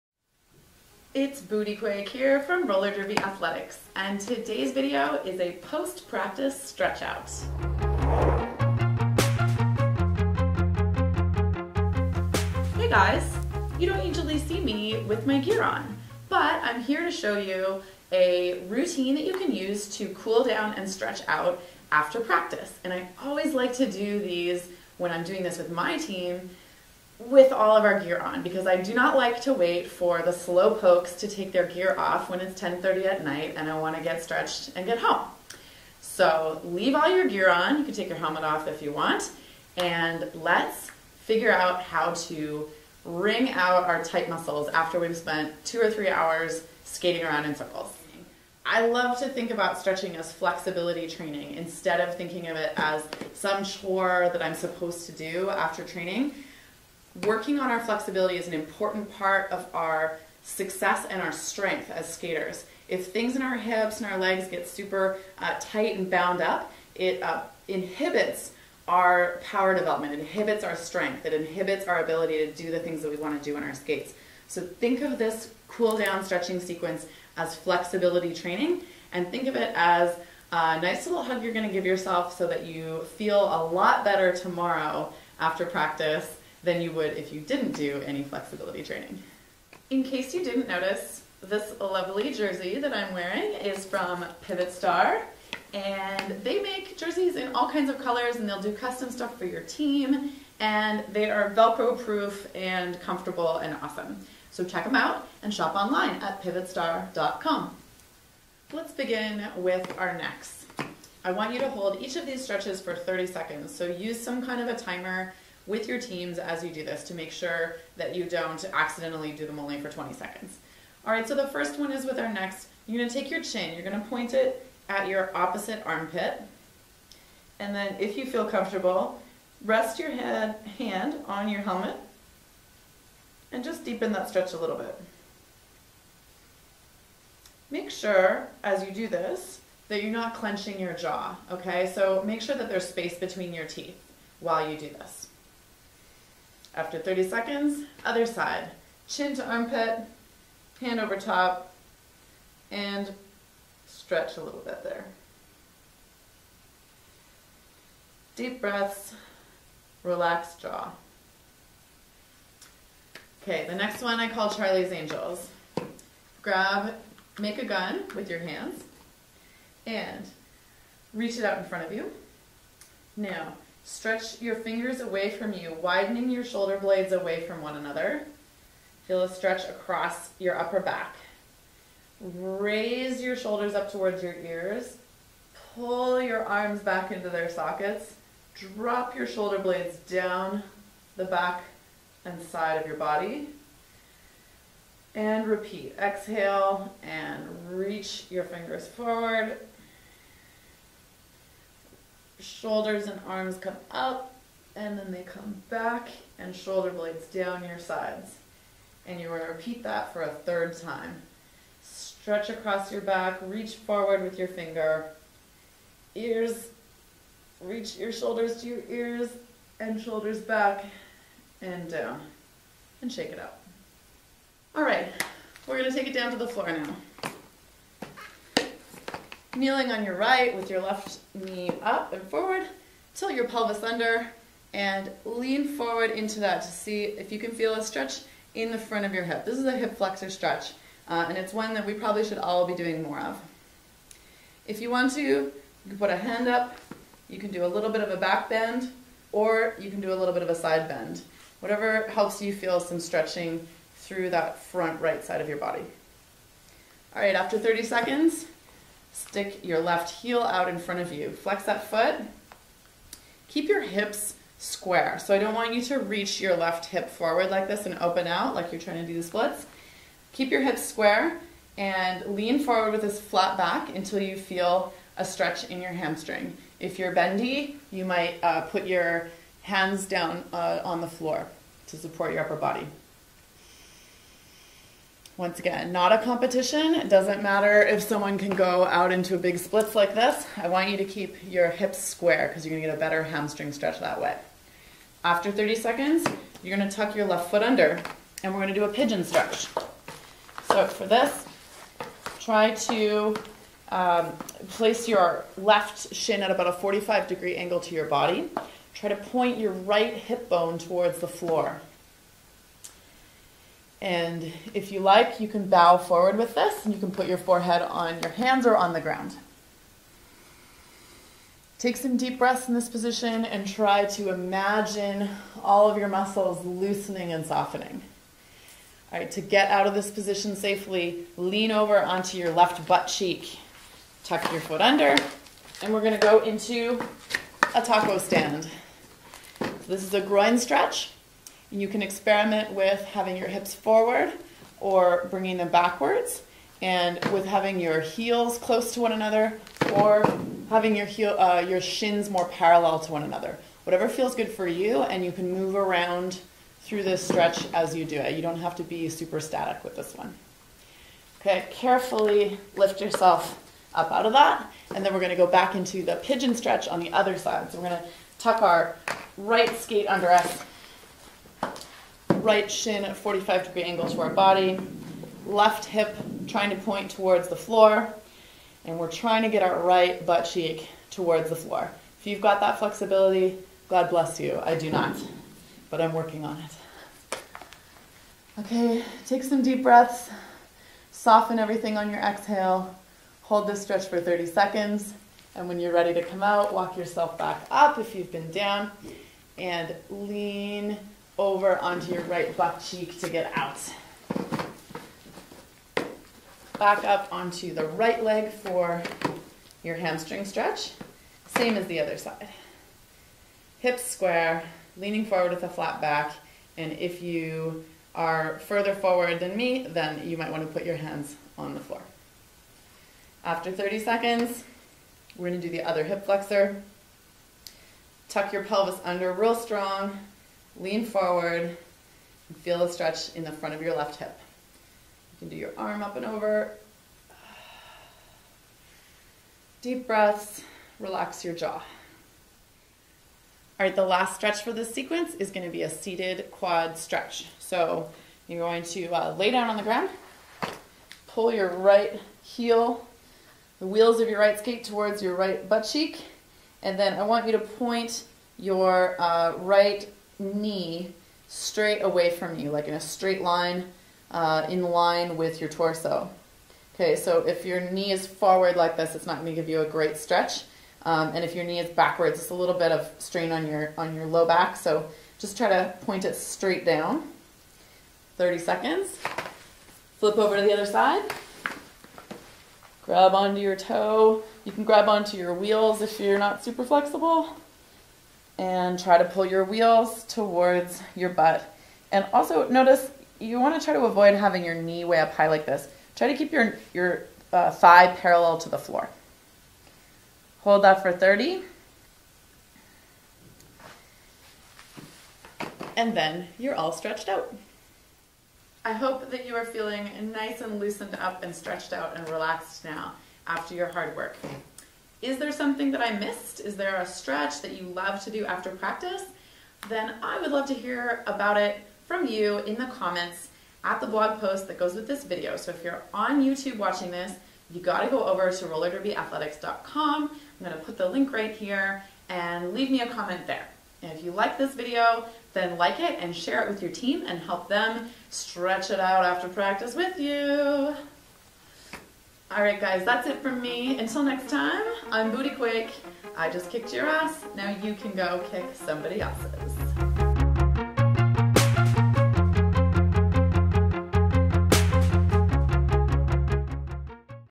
Stretching After Derby | Roller Derby Athletics
Incorporating flexibility training into your regular routine is a prime way to keep all the gears oiled and running smoothly. That’s why I’m sharing my Quick and Dirty Post-Derby Stretch (aka flexibility routine!): to give you a bare bones, full body flexibility sesh so you have no excuses for skipping it after a long practice! For full details, and to comment, check out the blog post that goes with this video